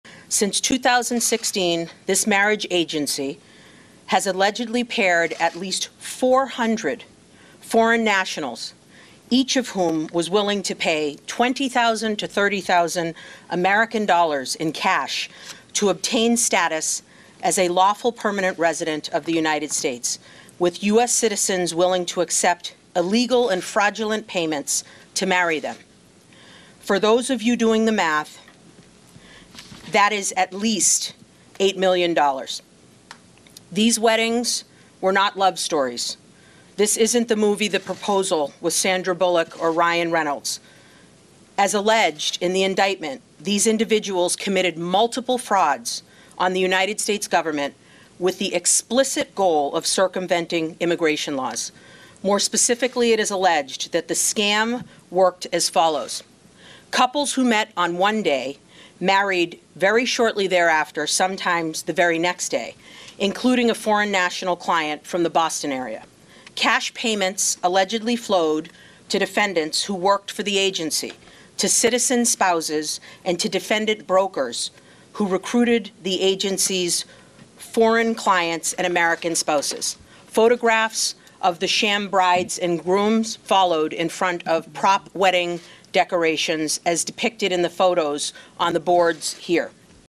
Rollins Announces Charges Involving LA-Based 'Sham' Marriage Agency
U.S. Attorney Rachael Rollins on Thursday announces charges involving a "sham" marriage agency based in Los Angeles.